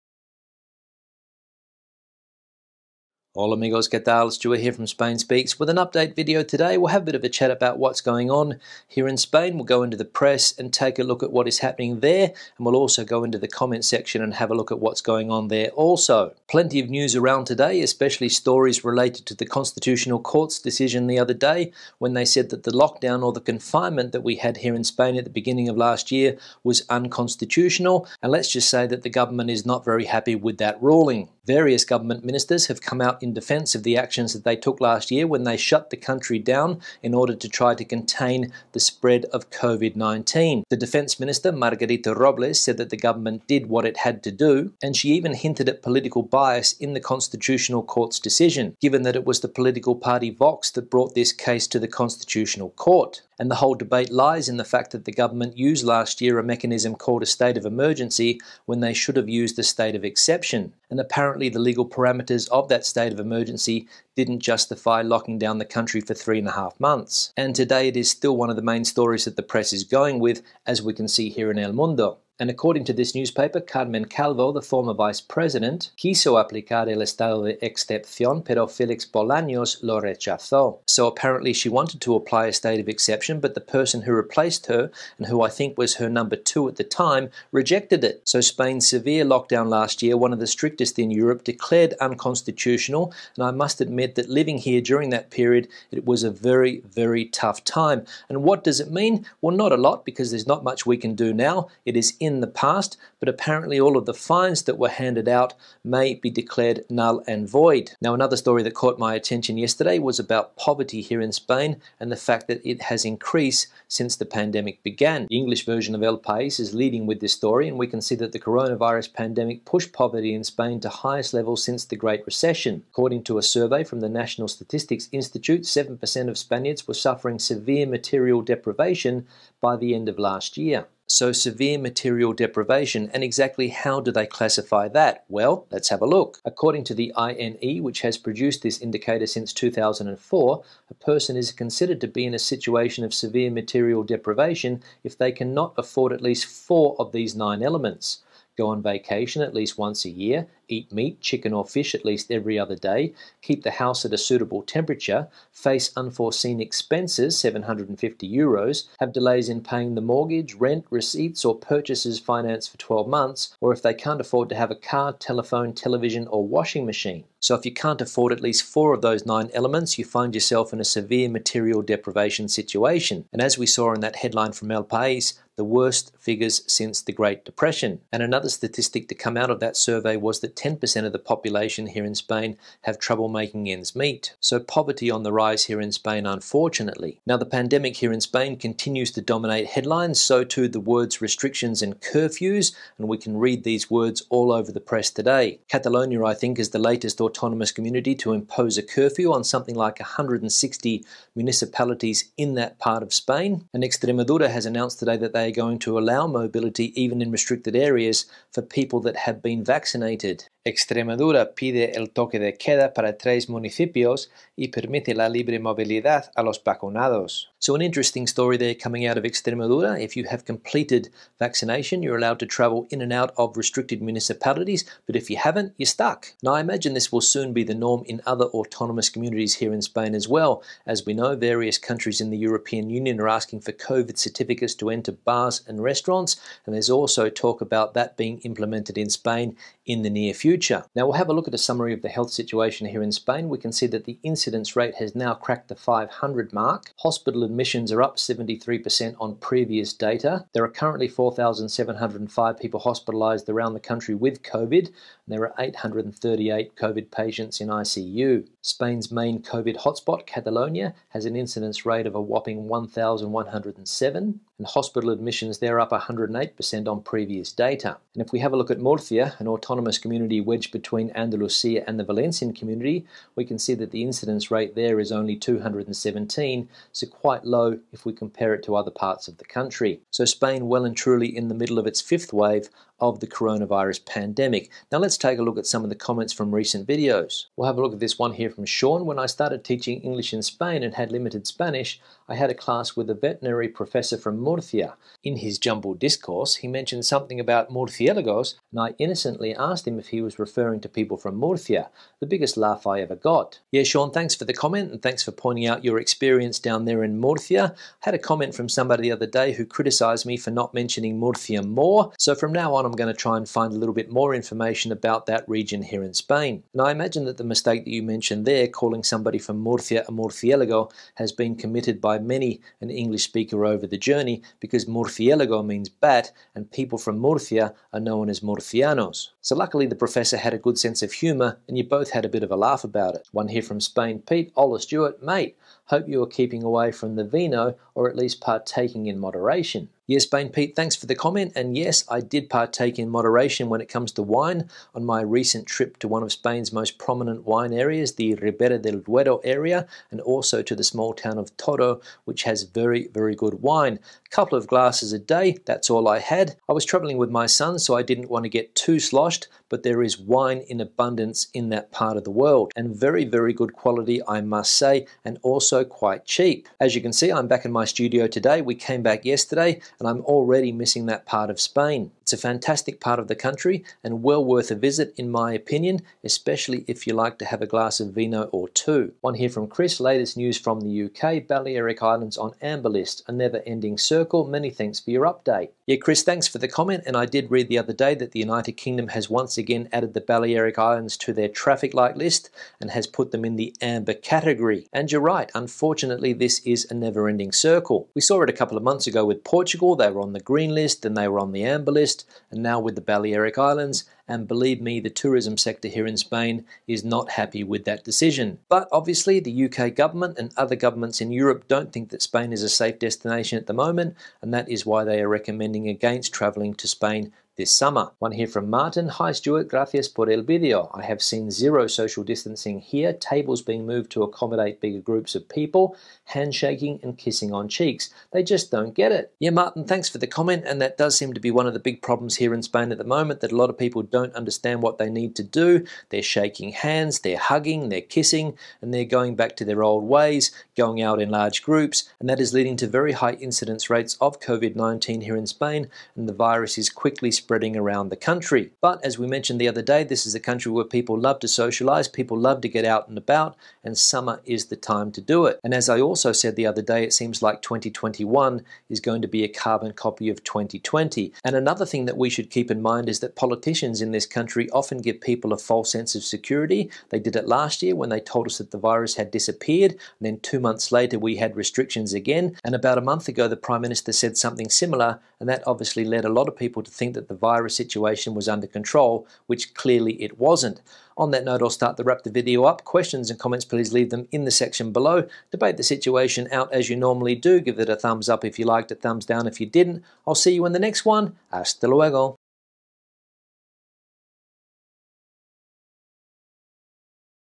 Spain update - A major screw up
Spain update and with the highest court in the land ruling that the lockdown in 2020 was unconstitutional, it seems the government has screwed up once again.

I film my videos on lumix cameras and lenses and sometimes on an iPhone.

I also create content for curious.com. Take my lessons +20,000 more @Curious on anything from tennis, to test prep, to tango.